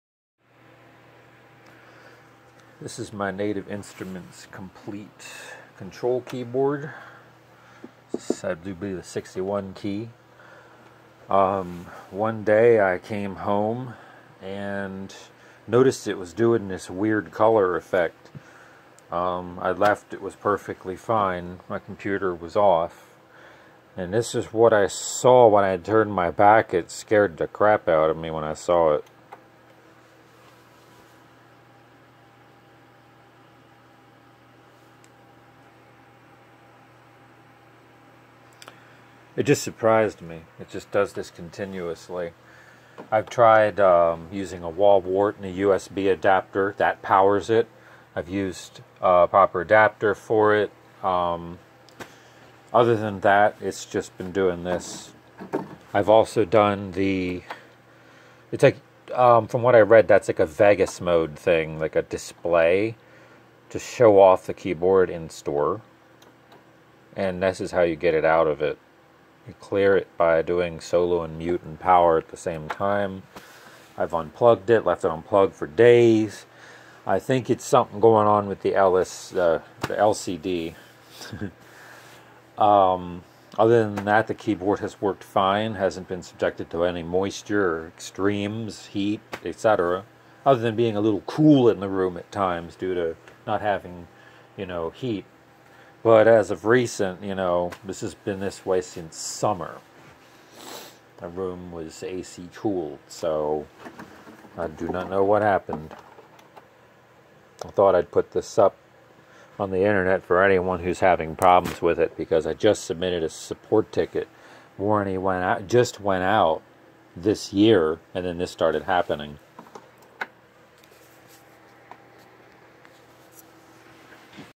Native Instruments Komplete Kontrol S61 (MKS2) Rainbow failure
I may be able to repair this but have not looked into it yet. I plan on trying to submit a support ticket. Over the summer of 2022 my only good MIDI keyboard had failed and began to do this :-(

My guess is that the LCD module is drawing excessive current leading to a continual loop. One thing I learned this looked like is Vegas mode. That is reset by powering on the unit while holding down S + M buttons both down

I uploaded this as a reference.